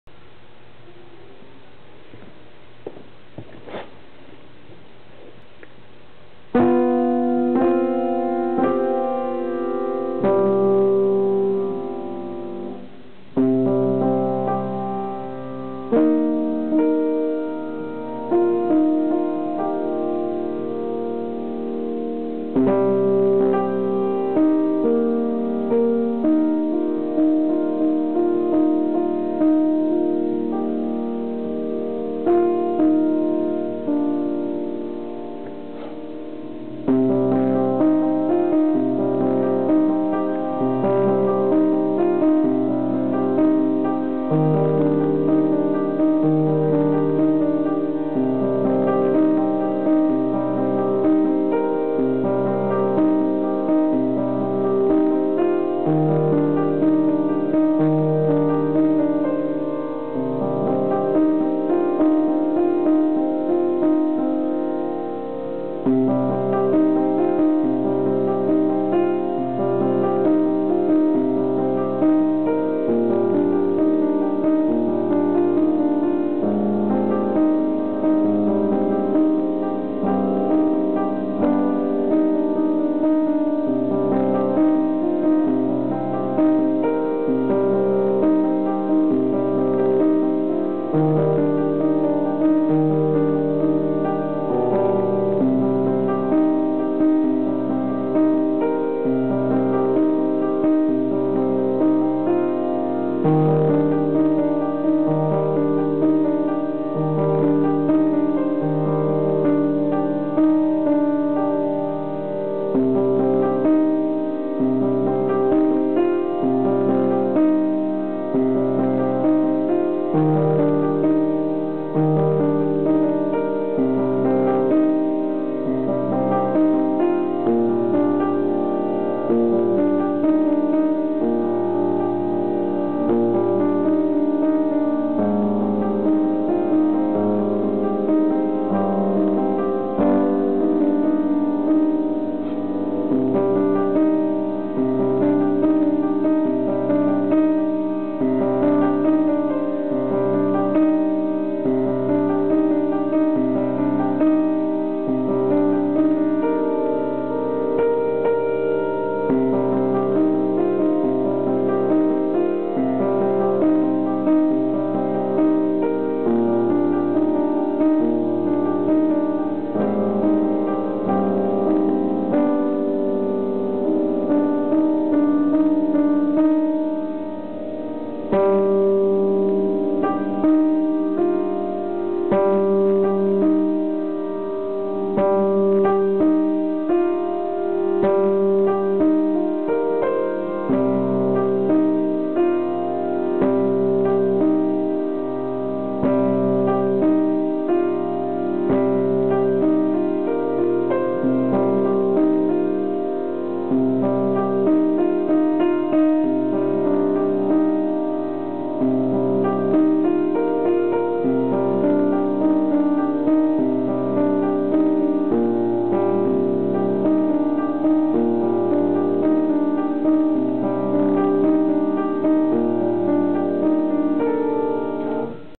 Piano improvisation
This video is very old, and I'm glad to be able to see improvements. I have no formal Piano background, I simply play by ear. I do realize I am no where near as talented like an actual Pianist so constructive criticism is always appreciated, emphasis on Constructive. I do mess up, but I'm learning each day, Spot the Tool